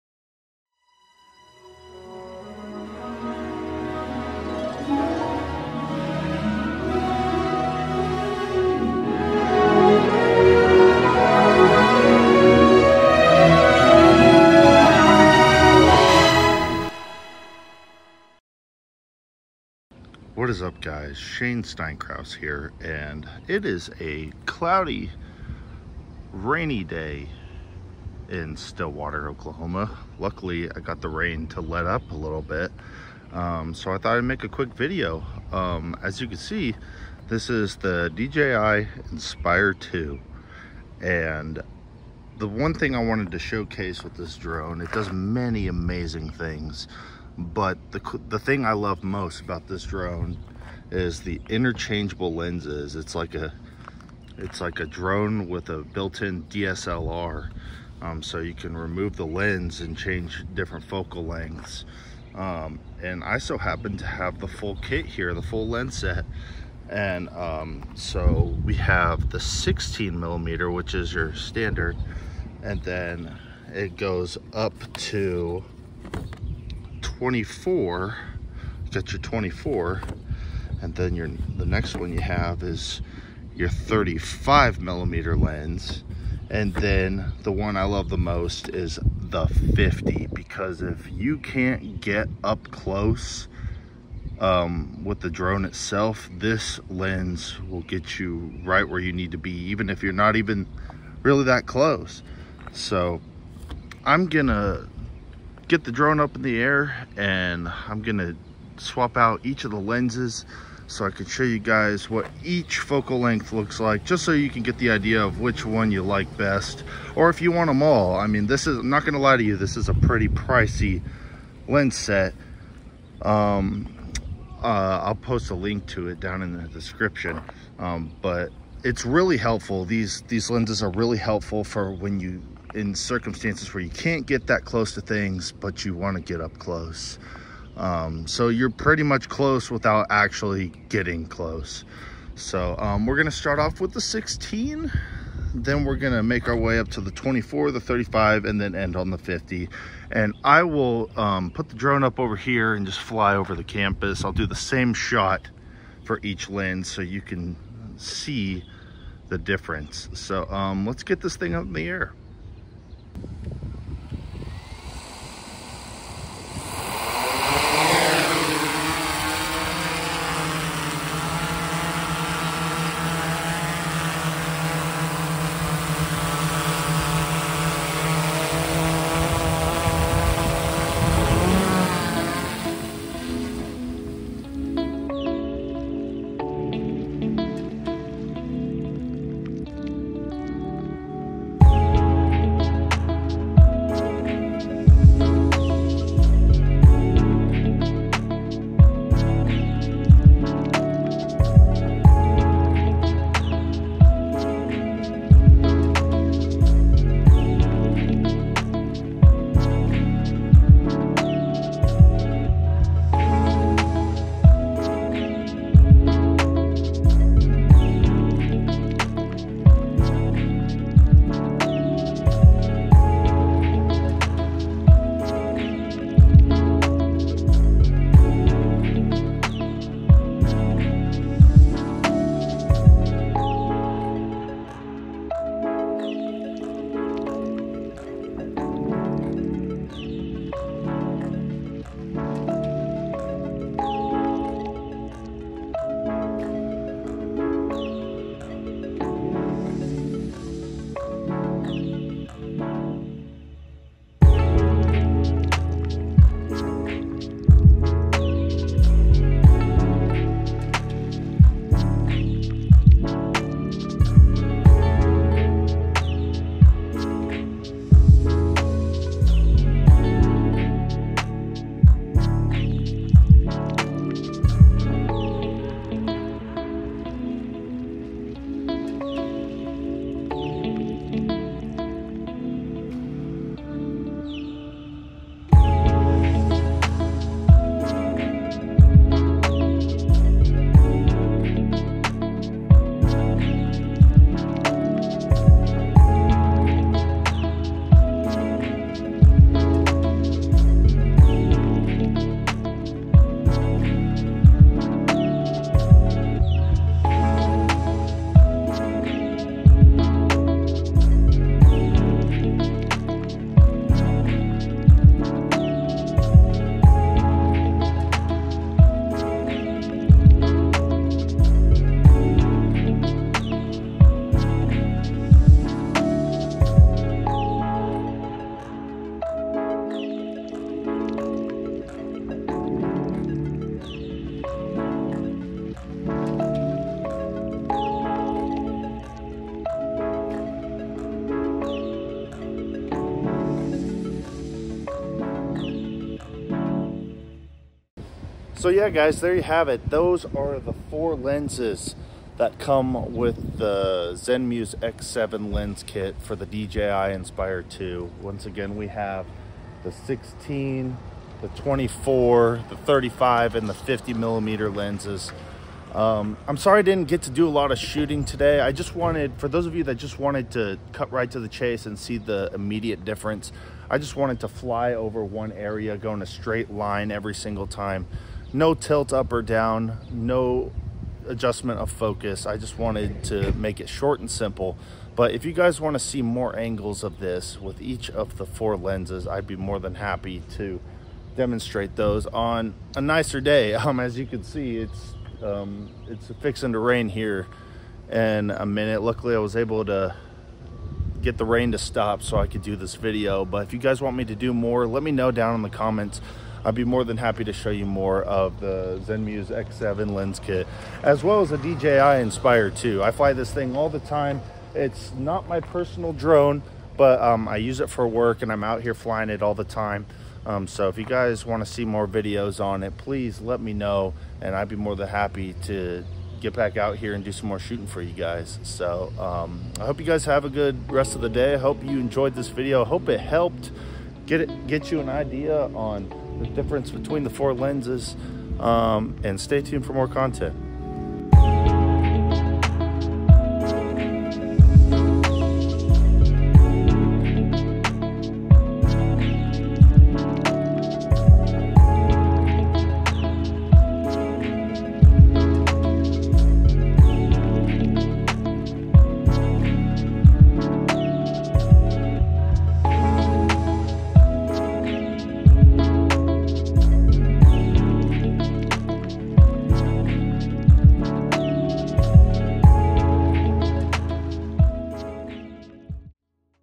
DJI Inspire 2: Zenmuse X7 Lens Comparison (16 mm, 24 mm, 35 mm, 50 mm)
Let me know if y'all would like to see more lens comparison videos. I do not own this drone, this drone is property of Oklahoma State University so I won't be able to post more in the event I get another job.

Lofi Geek Official Channel: @lofigeek
Artwork @mb0sco